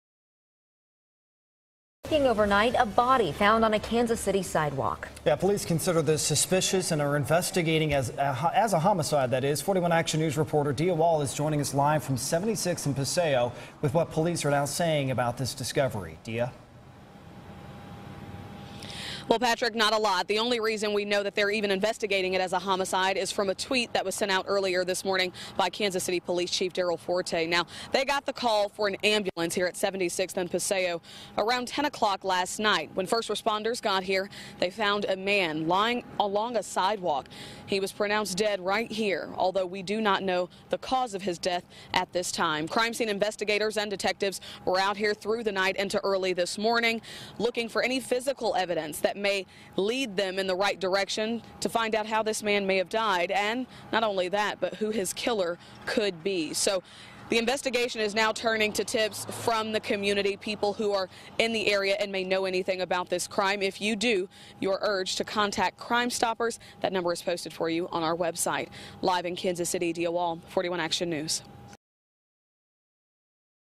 Police investigate homicide in south Kansas City
Police are investigating what they are calling a homicide after a man was found dead on a south Kansas City street Monday night.

41 Action News, KSHB, brings you the latest news, weather and investigative reports from both sides of the state line.